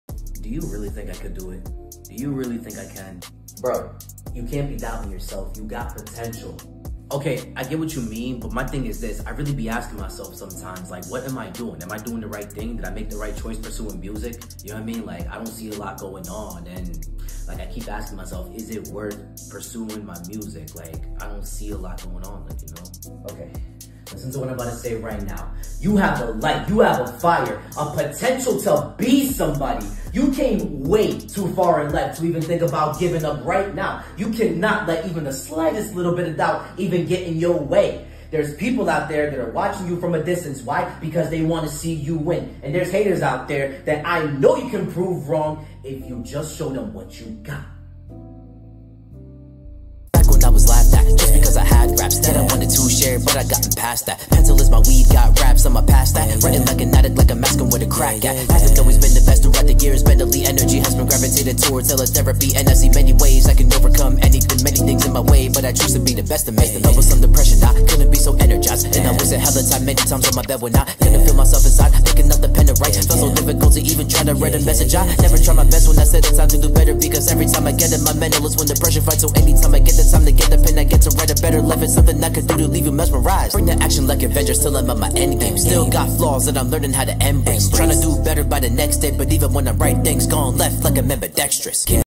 MP14 - How Far (Motivation Skit)
If you’ve ever felt this way, just know that you’re not the only one!

As an artist, I’ve had moments of doubt, worry, feeling lost. In the end, I remember to keep going!

Never doubt yourself. Always remember that you have a light, a fire, a potential. Never lose that light in you!